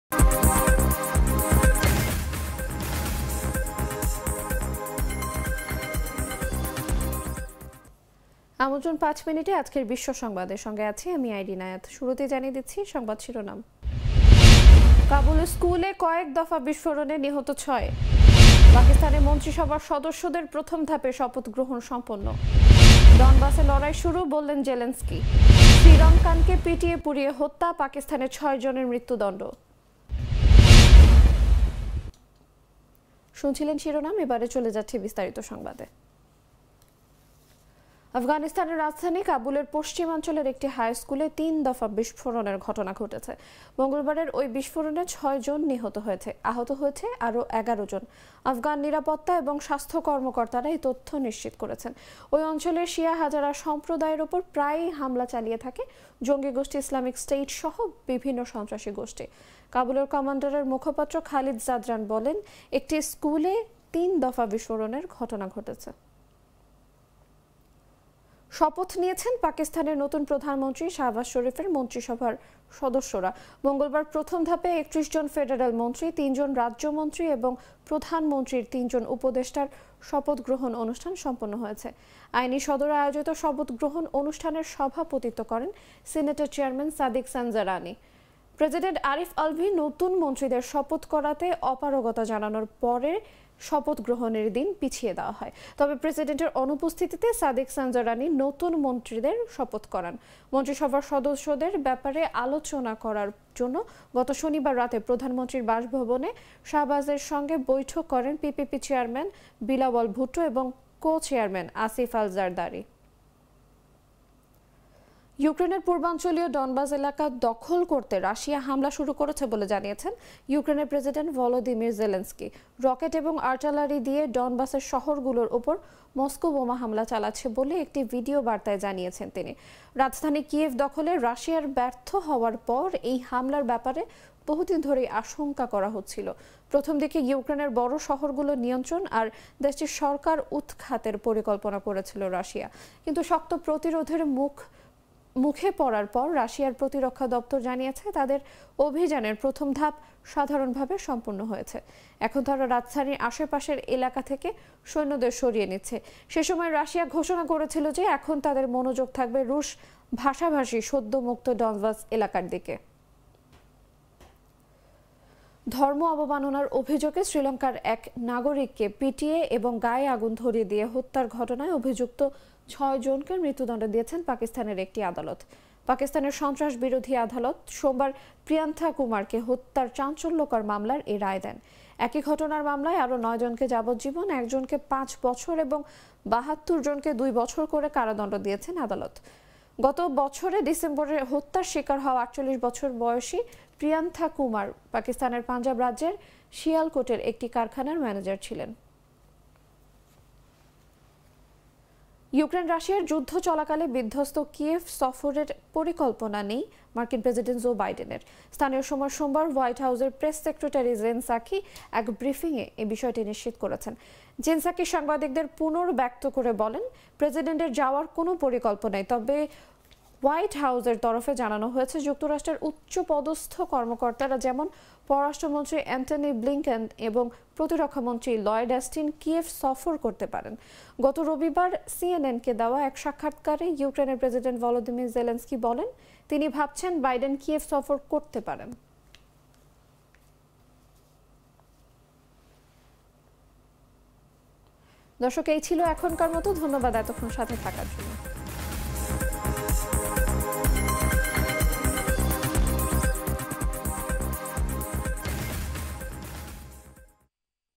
৫ মিনিটে আজকের বিশ্ব সংবাদ | ১৯ এপ্রিল ২০২২ || Jago News
৫ মিনিটে আজকের বিশ্ব সংবাদ | ১৯ এপ্রিল ২০২২

কাবুলে স্কুলে কয়েক দফা বিস্ফোরণে নিহত ৬

পাকিস্তানে মন্ত্রিসভার সদস্যদের প্রথম ধাপে শপথগ্রহণ সম্পন্ন

ডনবাসে লড়াই শুরু: জেলেনস্কি

শ্রীলঙ্কানকে পিটিয়ে-পুড়িয়ে হত্যা, পাকিস্তানে ৬ জনের মৃত্যুদণ্ড